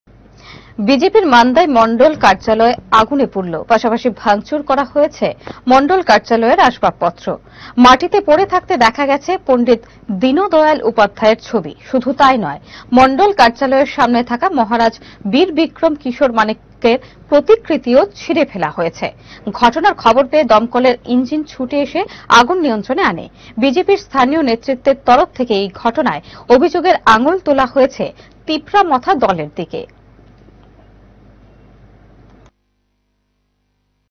বিজেপির মান্দাই মণ্ডল কার্যালয় আগুনে পুড়ল। পাশাপাশি ভাংচুর করা হয়েছে মণ্ডল কার্যালয়ের আসবাব পত্র।
বিজেপির মান্দাই মণ্ডল কার্যালয় আগুনে পুড়ল। পাশাপাশি ভাংচুর করা হয়েছে মণ্ডল কার্যালয়ের আসবাব পত্র। মাটিতে পরে থাকতে দেখা গেছে পণ্ডিত দীন দয়াল উপাধ্যায়ের ছবি। শুধু তাই নয় মণ্ডল কার্যালয়ের সামনে থাকা মহারাজ বীর বিক্রম কিশোর মাণিক্যের প্রতিকৃতিও ছিঁড়ে ফেলা হয়েছে । ঘটনার খবর পেয়ে দমকলের ইঞ্জিন ছুটে এসে আগুন নিয়ন্ত্রনে আনে । বিজেপির স্থানীয় নেতৃত্বের তরফ থেকে এই ঘটনায় অভিযোগের আঙ্গুল তোলা হয়েছে তিপ্রা মথা দল আশ্রিত দুষ্কৃতিদের দিকে।